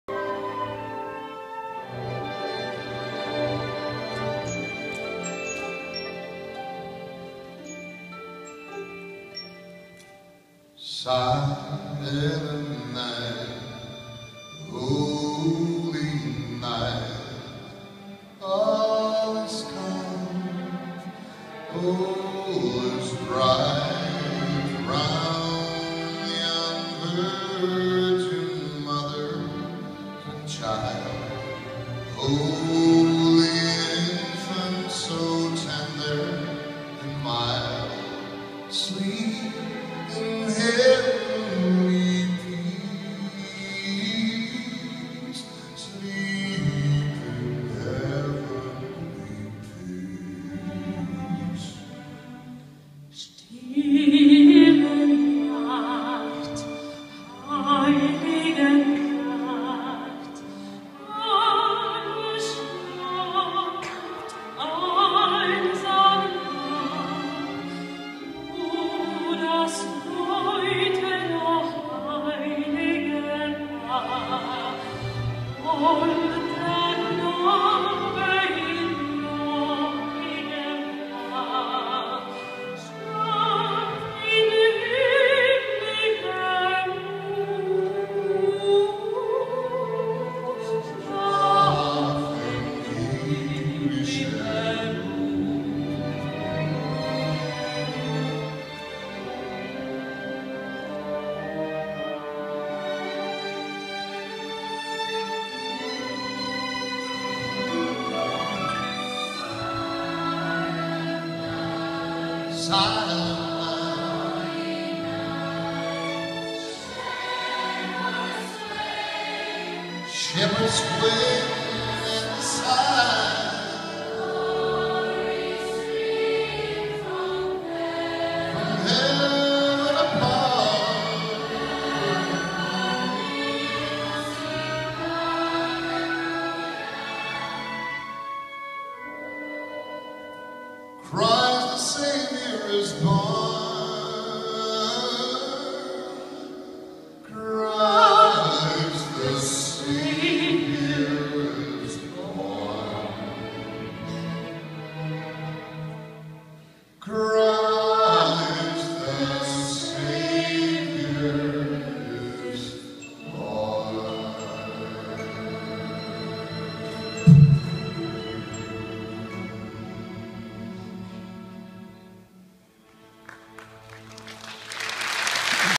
Коледен концерт 2014
Коледен концерт във ФКЦ-Варна, гост Васил Петров